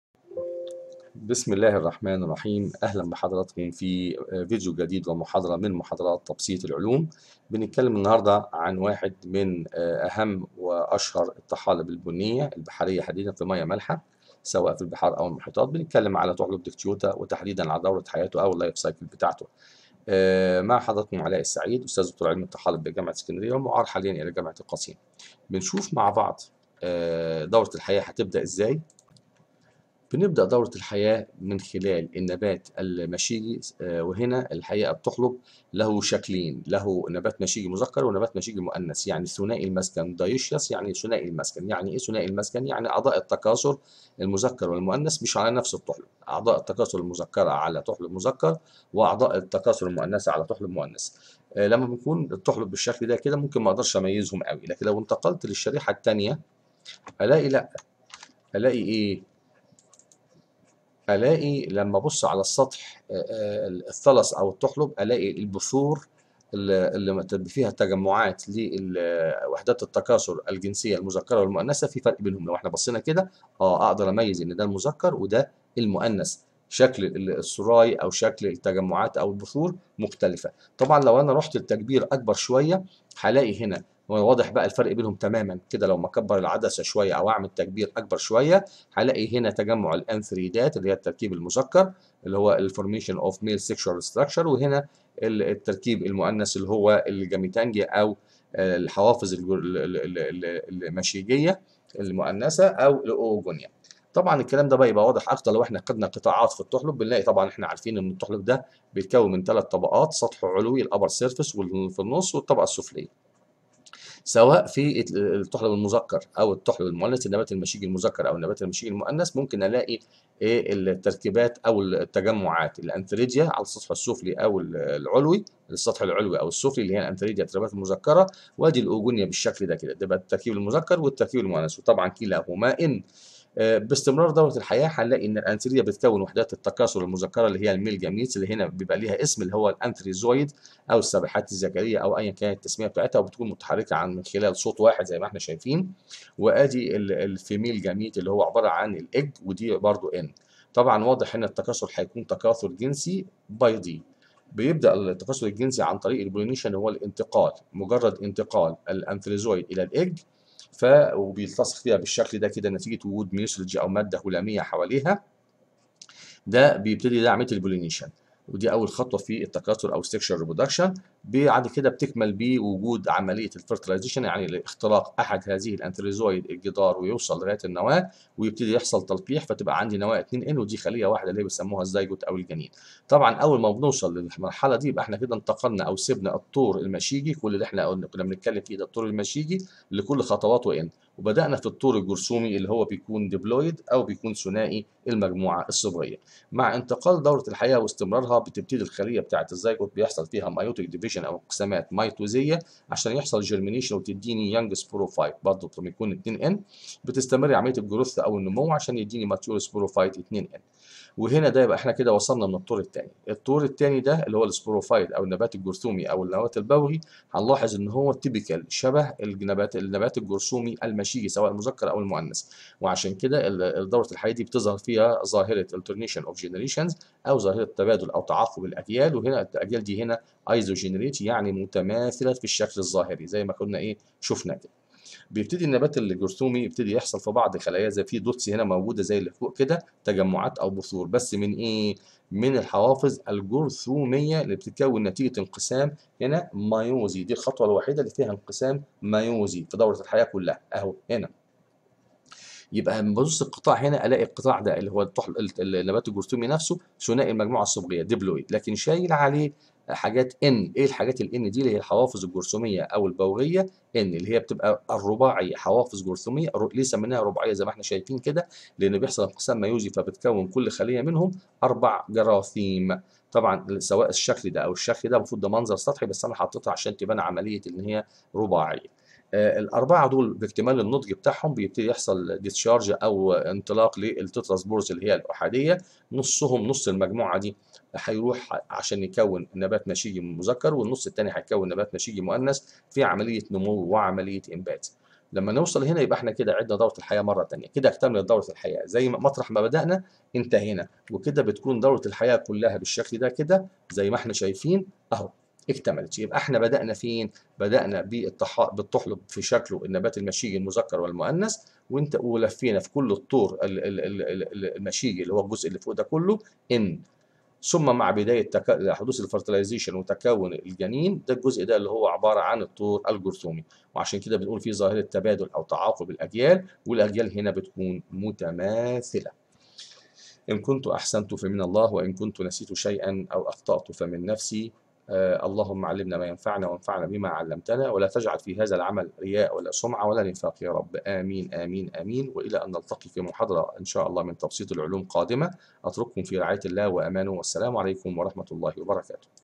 Dictyota life cycle
شرح مبسط و مختصر لدورة حياة الطحلب البني داكتيوتا